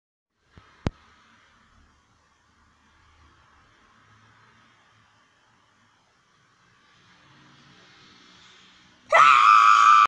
space onion goes to earth
made by a species of potato that made humanity go extinct and now they rule the world. But it doesn't end there, because now they are spreading all over the world and soon everything will be "potato".